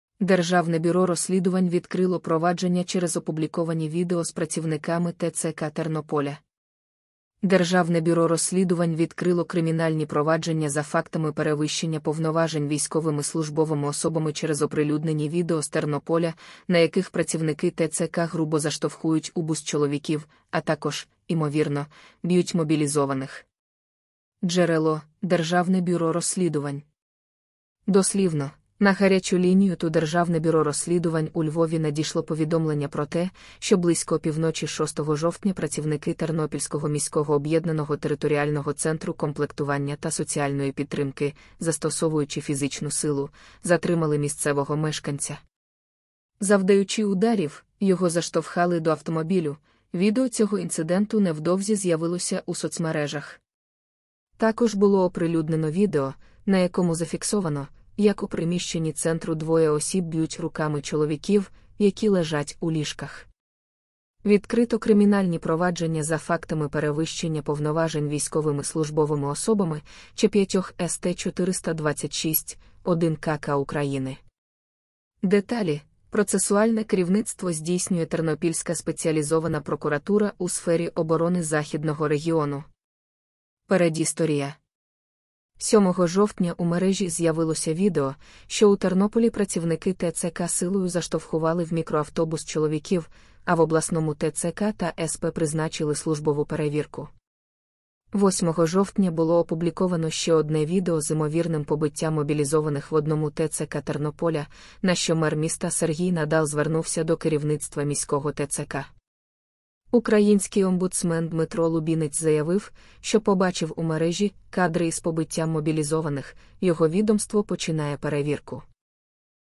ДБР відкрило провадження через опубліковані відео з працівниками ТЦК Тернополя
Наш онлайн курс Pro Військове право

In this eye-opening video, I share the shocking details of the SBI proceedings against the Ternopil TCC. The recently published videos showcasing the behavior of the employees have caught the attention of authorities. Join me as I delve into the investigation, uncovering the truth behind the controversy. Get ready to be informed and amazed by the revealing footage that has stirred up a storm. Don't miss out on this compelling story that will leave you questioning the integrity of the Ternopil TCC. Watch now to stay in the know!